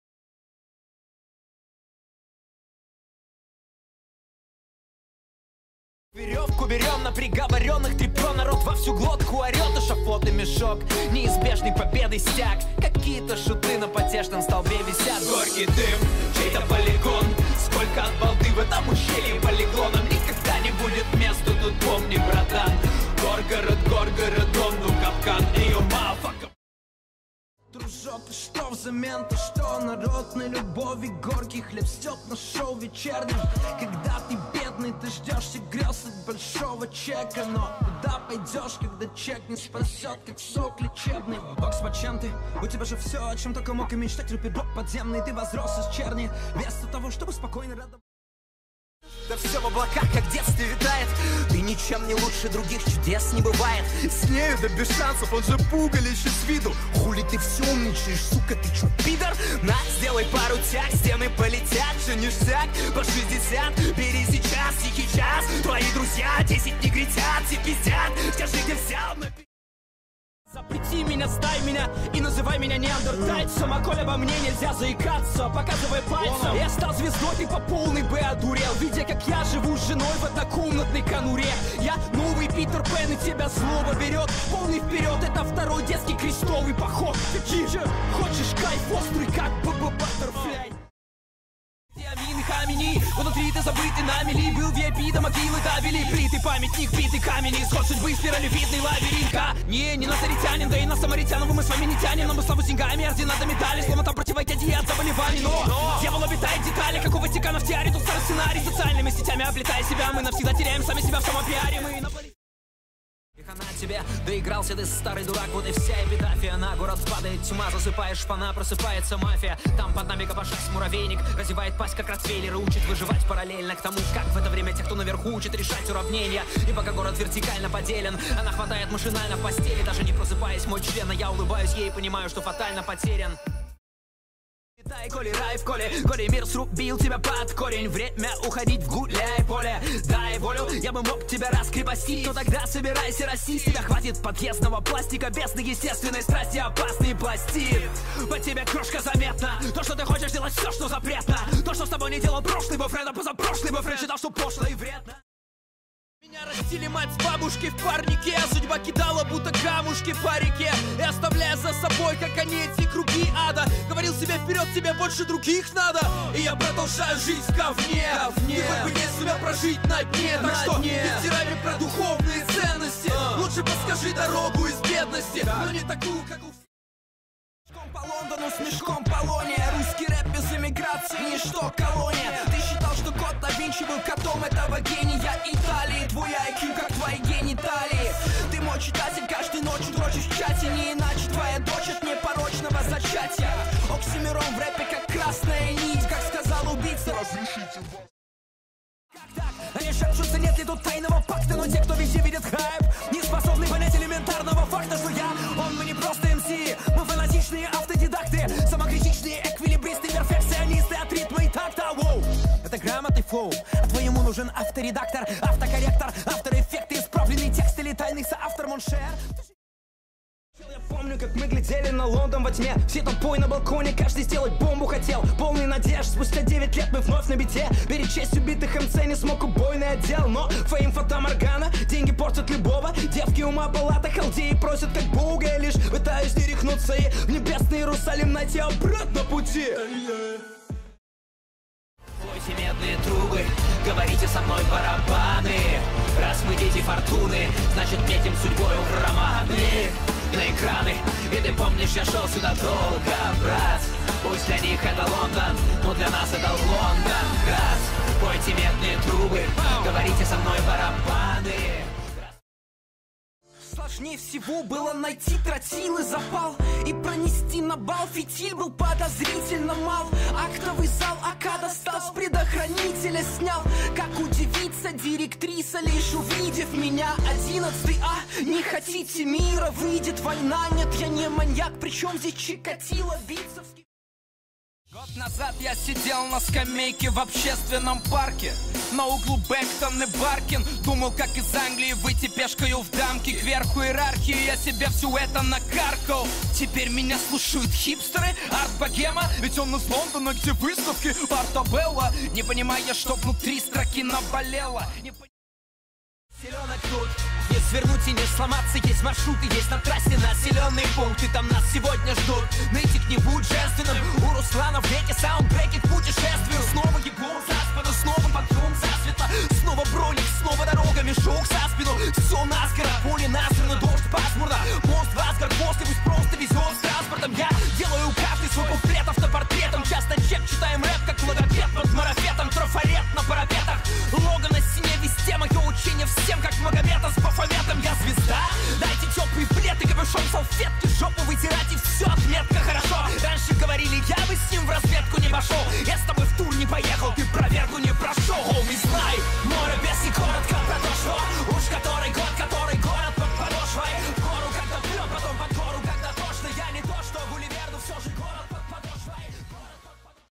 ПОПРОБУЙ НЕ ПОДПЕВАТЬ ТРЕКИ ОКСИМИРОНА.
Это видео отличается от остальных. Теперь ты должен не угадывать песни, а пытаться не подпевать. В данном челлендже песни сложные, поэтому я сомневаюсь, что сможешь пройти. Но если вдруг прошёл, то обязательно поделись этим в комменты, это должны знать все. Также можешь посоревноваться со своими друзьями, просто пришли им это видео, и узнай сколько они продержались.
Также можешь поддержать меня своим лайком и подпиской, скоро будут еще подобные видео. Не пропусти!